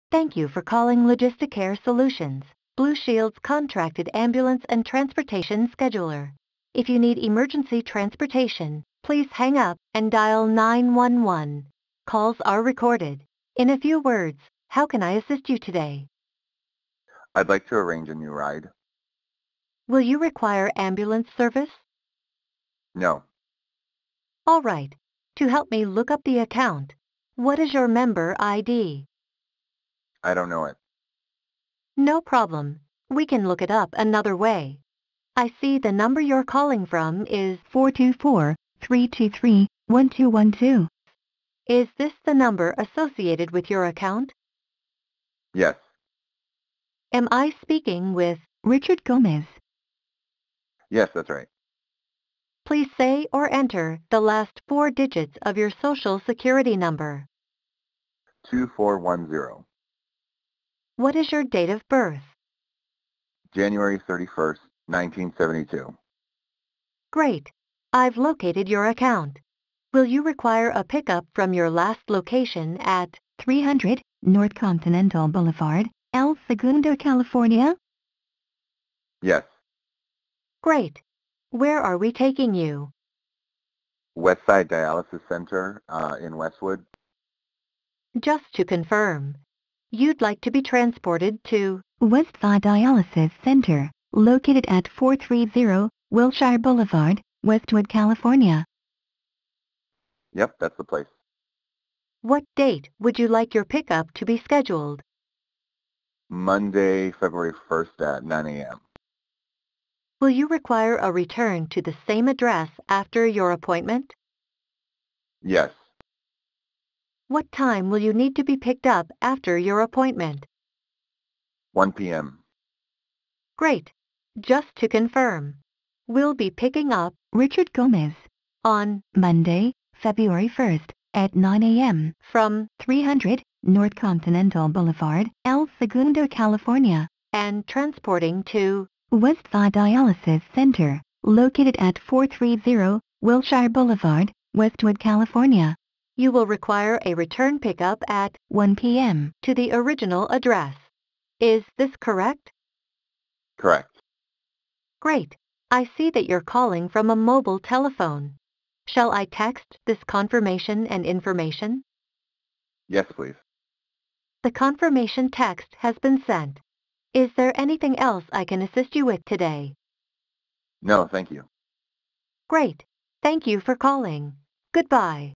Transportation — Logisticare — Paratransit: Schedule a Ride with an AI Virtual Agent
In this realtime call, conversational AI assists a caller with scheduling a paratransit ride. Using natural language, AI virtual agents can capture intent and authenticate a customer through ANI lookup and a few pieces of identifying information. Through behavioral data from system of record integration, intelligent virtual agents can pull up the last ride and provide details to save the caller time.